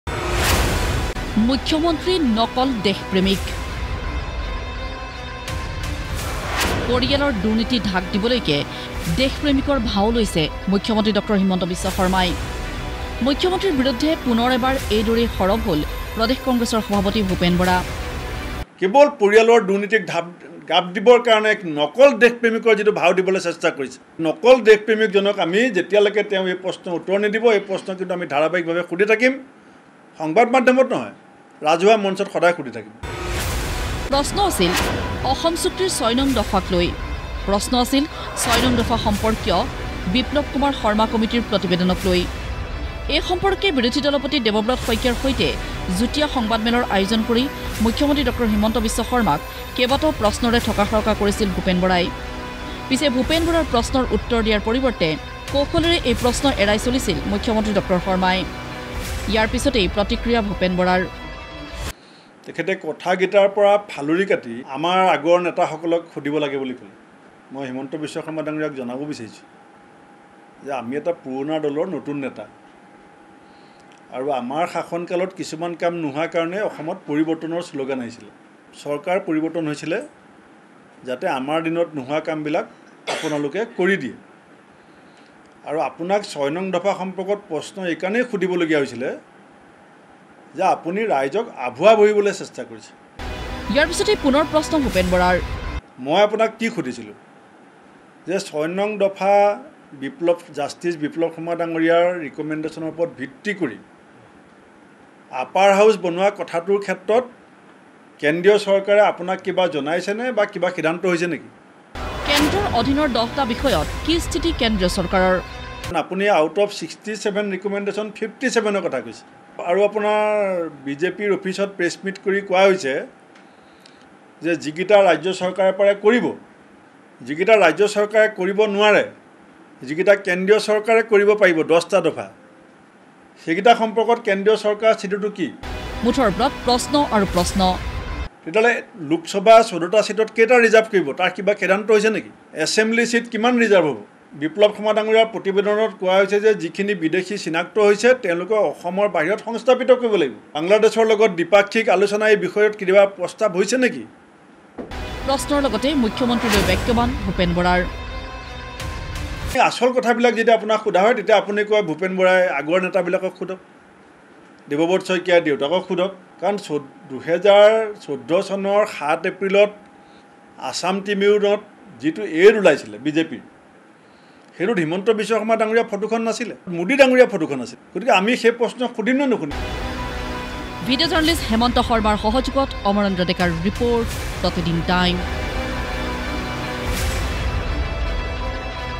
মুখ্যমন্ত্রী ড০ হিমন্ত বিশ্ব শৰ্মাক নকল দেশপ্রেমিক বুলি ক'লে কোনে?
মুখ্যমন্ত্রী ড০ হিমন্ত বিশ্ব শৰ্মা নকল দেশপ্রেমিক। পৰিয়ালৰ দুৰ্নীতি ঢাকিবলৈকে দেশপ্রেমিকৰ ভাও জুৰিছে মুখ্যমন্ত্রী ড০ হিমন্ত বিশ্ব শৰ্মাই। এই অভিযোগ প্রদেশ কংগ্রেছৰ সভাপতি ভূপেন বৰাৰ। ৬ নম্বৰ দফা সম্পৰ্কীয় বিপ্লৱ কুমাৰ শৰ্মা সমিতিৰ প্রতিবেদনক লৈ কংগ্রেছে উত্থাপন কৰা প্রশ্নৰ পৰা ফালৰি কটাৰ অভিযোগ তুলি মুখ্যমন্ত্রীৰ বিৰুদ্ধে এনেদৰে সৰৱ হৈ পৰিছে প্রদেশ কংগ্রেছৰ সভাপতিগৰাকী।